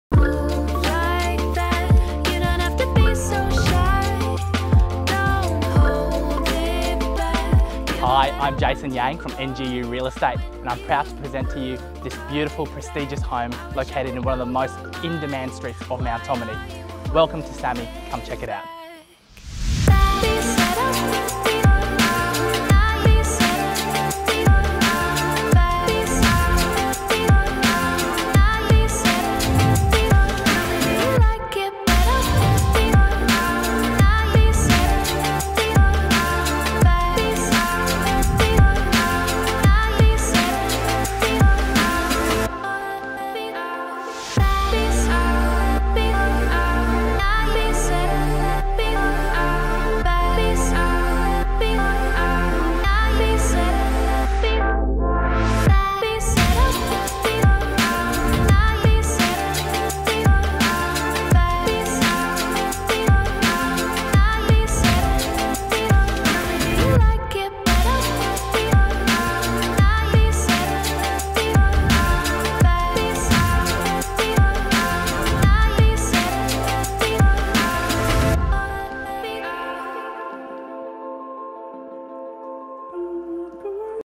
4 Drysdale Street, Mount Ommaney - Presented By Jason Yang
‘Samy’, grand in its proportions, with an impressive street presence, is a custom-built residence which embodies timeless elegance. Occupying a substantial 1016m2 flat allotment at a prized Mount Ommaney address, this dual level family entertainer presents a flexible layout, multiple living areas and sweeping elevated views over an adjacent golf course, its lakes and to mountains beyond.

The downstairs area comprises of a grand layout offering separate formal and informal lounge and dining areas with majestic views of the private pool. These areas are enhanced by character tiles, new quality carpets and high ceilings. The kitchen is superbly equipped and features a gas cooktop, dishwasher, and a large butler's pantry.

Family excellence continues with the home’s abundant upper-level and one lower-level accommodation. All five king sized bedrooms are appointed with floor-to-ceiling built-in wardrobes two of which have their own bathroom. The palatial master bedroom enjoys a walk-through dressing room with extensive built-in wardrobe space plus a full sized ensuite with a standalone spa bath, shower and his and hers’ basins. Further highlights of this stunning residence include a formal lounge/media room, quality gas fireplaces in both the formal and informal lounge/dining areas, ducted air conditioning and video security intercom.

The residence is complete with an extensive amount of storage plus a double automatic garage and off-street parking for a further three cars. This beautiful family home enjoys a tightly held address, offering easy access to prestige private schools. It is moments away from many conveniences such as the Mount Ommaney shopping centre, with a surplus of restaurants and shopping options, Jindalee Reading Cinema, the Home Base Centre with DFO, two 18-hole golf courses and a local boat ramp for water sports or fishing.

Key Features of this stunning home include:
- 5 king sized bedrooms
- 4 bathrooms
- Grand entrance with a full height foyer
- Spacious lockup garage
- Recently upgraded ducted air-conditioning
- Extensive outdoor entertaining areas
- Security system with remote gate entry
- Intercom
- Spacious laundry with direct outdoor access
- Fully fenced
- Rainwater tank with integrated pressure pump system

First time offered to the market, this is your unique opportunity to call this exclusive Mt Ommaney address home, get in touch with your local licensed real estate agent – Jason Yang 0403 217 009 to arrange an inspection today. Properties like this do not last.